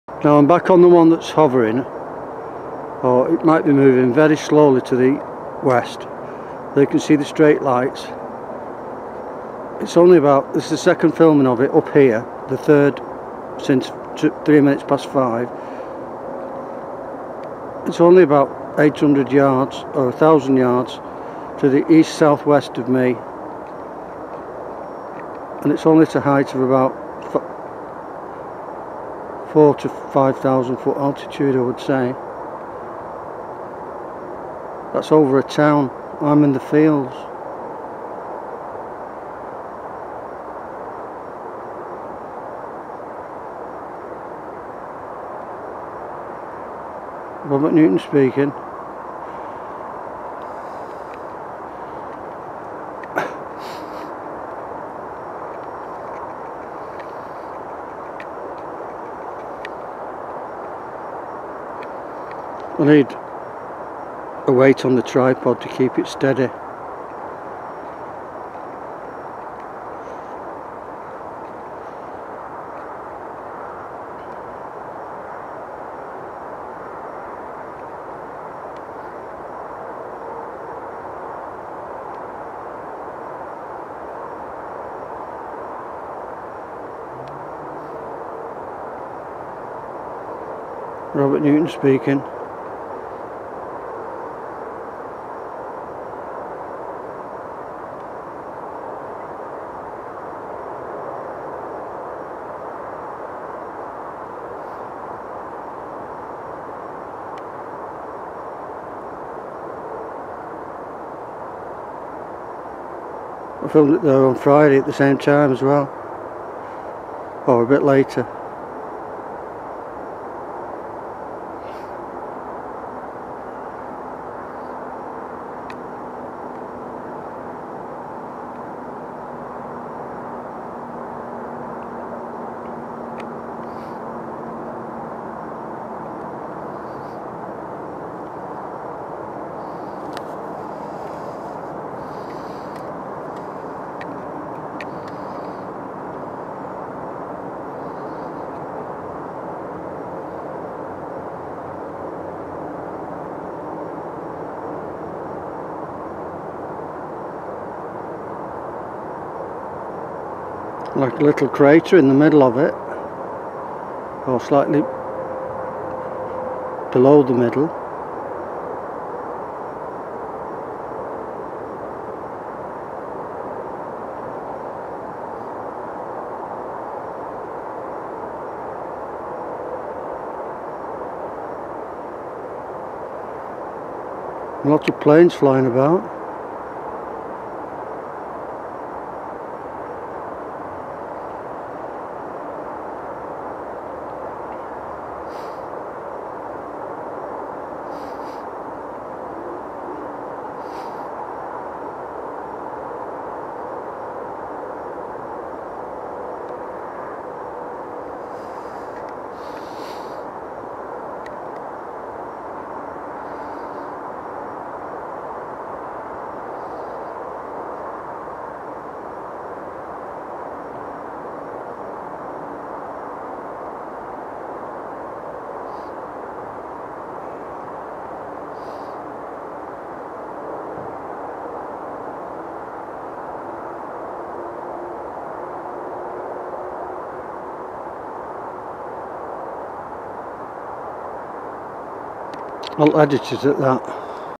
Alien or interdimensional craft or entity!
Mon.28.nov.2016,about 5-28 pm! Or is it Venus? Robert Newton.Britain.PS.Pleidian? PS. If not Venus then under 1.5 mls.,to,ESW,about 8,000 ft.,alt! Planet's don't change colour!